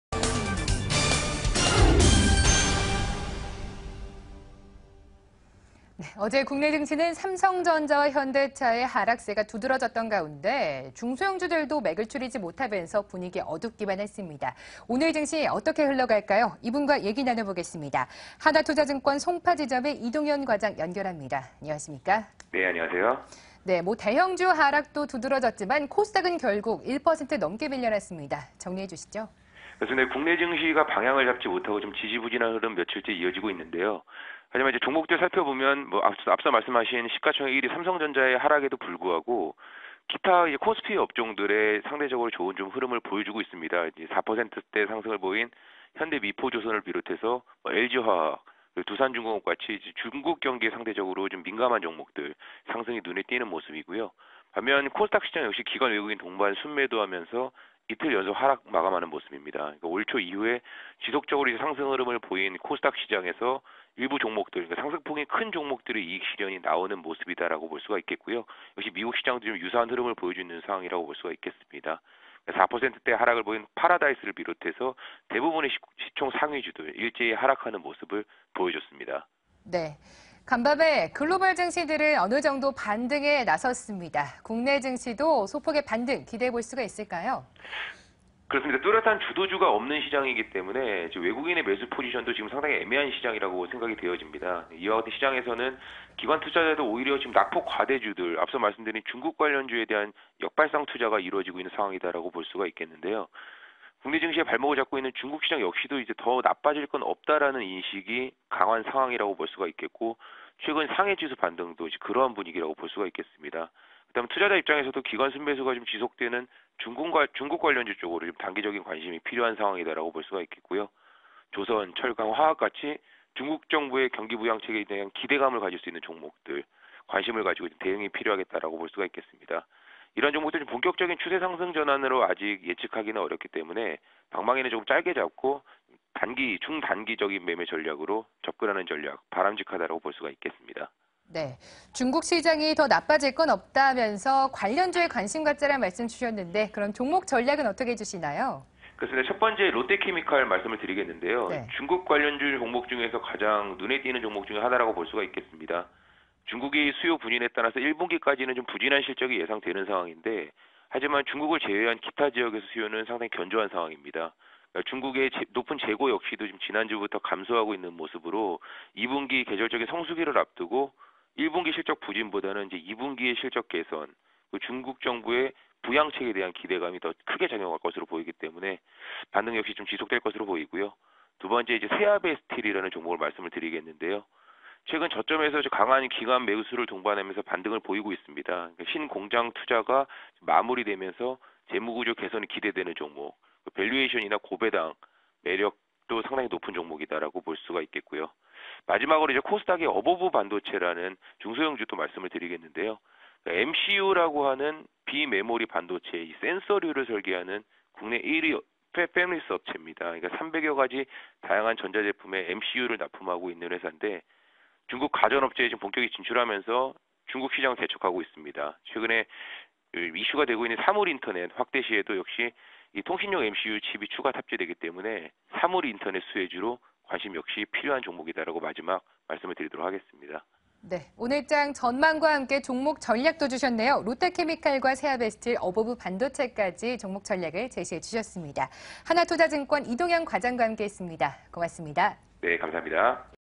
코스피 안개속 장세 지속.. 전략은?
Q. G2 우려로 1940선 '턱걸이'.. 해석은?

- 삼성전자 하락 감안 상대적 선방
 - 中 경기관련주 상승 뚜렷
 - 코스닥, 美 증시 동행→시총상위주 하락

 Q. 안개속 장세 지속.. 전략은?

- 기관 매수 종목 단기 트레이딩
 - 기관, 낙폭과대주 역발상 투자 주목
 - 中 시장 바닥권 인식 강화
 - 기관 매수 中 관련주 단기 관심
 - 상승추세 미전환→단기 매매

Q. 오늘 장 관심종목은?

- 롯데케미칼: 이익개선, 中 부양책 기대
 - 세아베스틸: 신공장 투자 마무리 / 고배당+저평가
 - 어보브반도체: 사물인터넷 확대 수혜